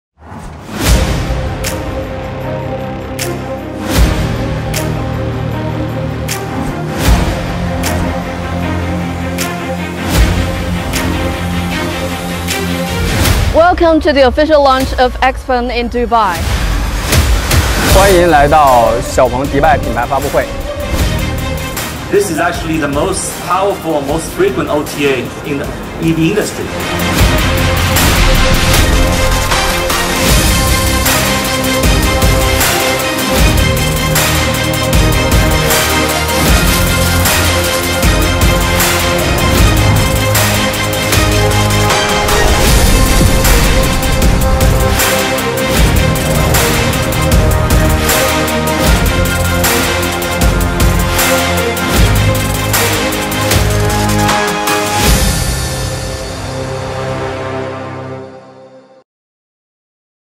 XPENG greets the UAE!
Highlights from the press event and exhibits in Dubai as XPENG enters the UAE market! 🇦🇪